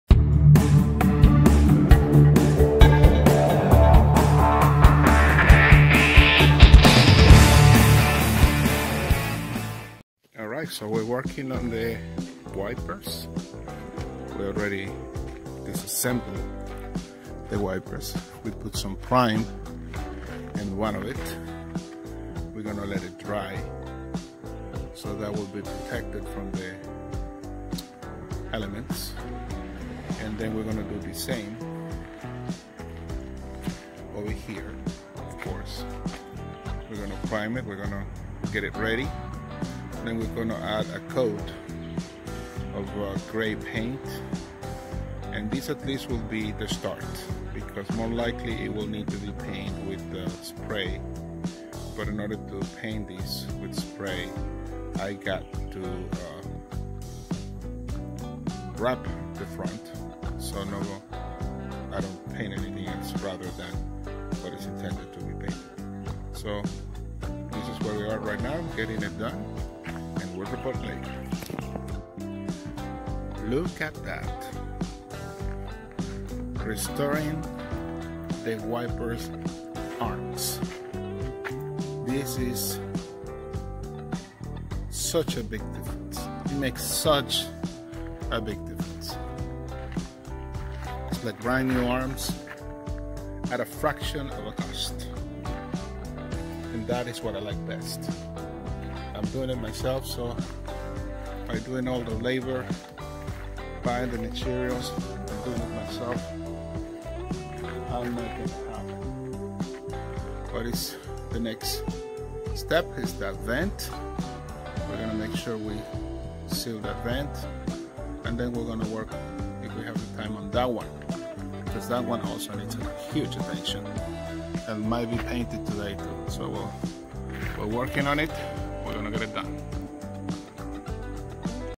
Refurbishing the windshield wipers arms, GMC Motorhome the Yaryis adventures part 47
This is part 2 of our wipers replacement and refurbishing sanding, painting, replacing and getting them back to brand new conditions at a fraction of the cost, that is great news in my book.

Follow our Yaryis Adventures